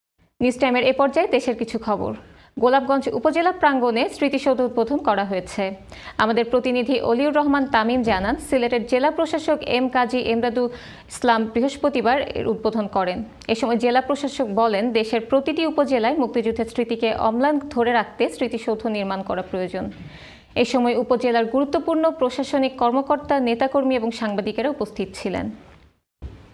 গোলাপগঞ্জ উপজেলা পরিষদ প্রাঙ্গনে বীর সৌধ উদ্বোধন করা হয়েছে।
গোলাপগঞ্জ উপজেলা পরিষদ প্রাঙ্গনে বীর সৌধ উদ্বোধন করা হয়েছে। আমাদের প্রতিনিধি অলিউর রহমান তামিম জানান, সিলেটের জেলা প্রশাসক এম কাজী এমদাদুল ইসলাম বৃহস্পতিবার এর উদ্বোধন করেন। এসময় জেলা প্রশাসক বলেন, দেশের প্রতিটি উপজেলায় মুক্তিযুদ্ধের স্মৃতিকে অম্লান করে ধরে রাখতে স্মৃতি সৌধ নির্মাণ করা প্রয়োজন। এসময় উপজেলার গুরুত্বপুর্ন প্রশাসনিক কর্মকর্তা, নেতাকর্মি এবং সাংবাদিকেরা উপস্থিত ছিলেন।
iON TV
SKY 782